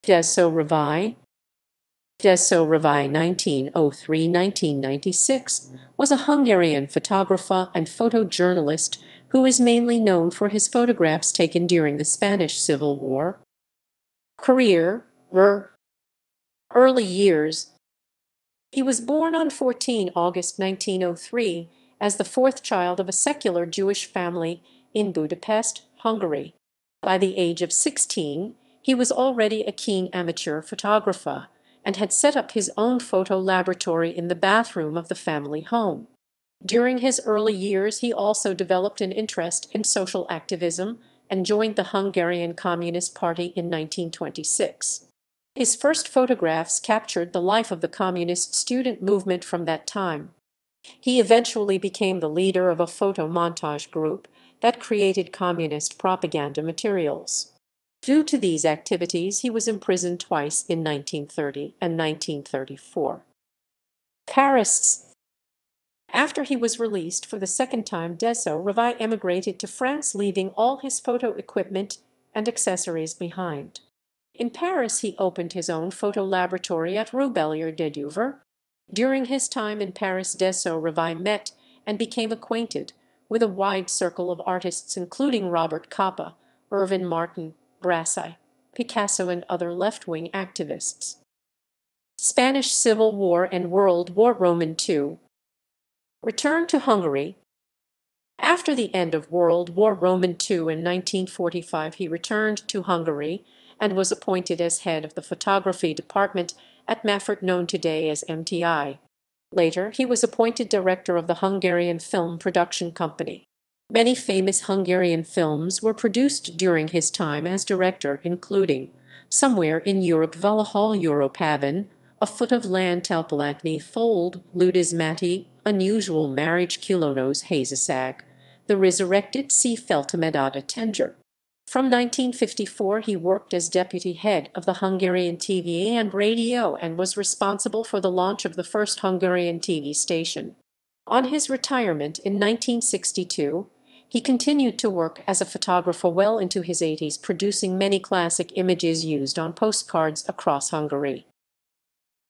Dezső Révai
Dezső Révai (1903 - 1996) was a Hungarian photographer and photo journalist who is mainly known for his photographs taken during the Spanish Civil War.